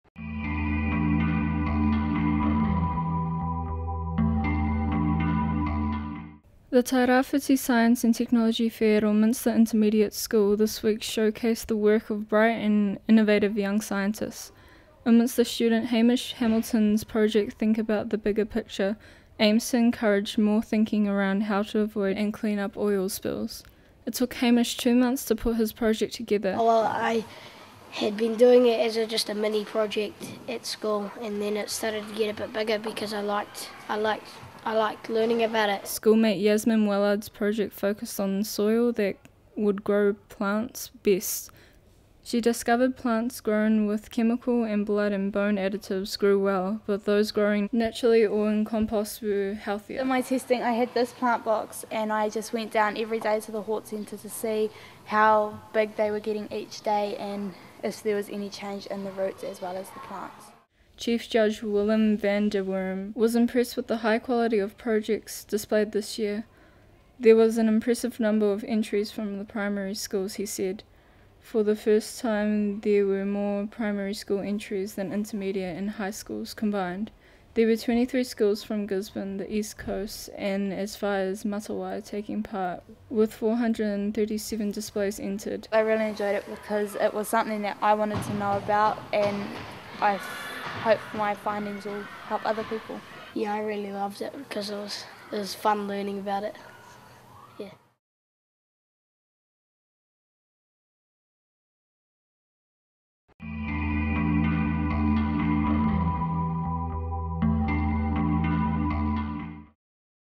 Tairawhiti Science and Technology Fair
THE Tairawhiti Science and Technology Fair at Ilminster Intermediate School this week showcased the work of bright and innovative young scientists.Ilminster student Hamish Hamilton’s project Think About the Bigger Picture aims to encourage more thinking around how to avoid and clean up oil spills.

It took Hamish two months to put his project together. Schoolmate Yasmin Wellard’s project focused on soil that would grow plants best. She discovered plants grown with chemical and blood and bone additives grew well, but those growing naturally or in compost were healthier. Event co-ordinator Shannon Kendall said there were slightly fewer entries than last year but the quality of work was very good. Chief judge Willem van den Worm was impressed with the high quality of projects displayed this year. There was an “impressive number” of entries from the primary schools, he said. “For the first time there were moreb primary school entries than intermediate and high schools combined.” There were 23 schools from Gisborne,the East Coast and as far as Matawai taking part, with 437 displays entered.

Ben Cowper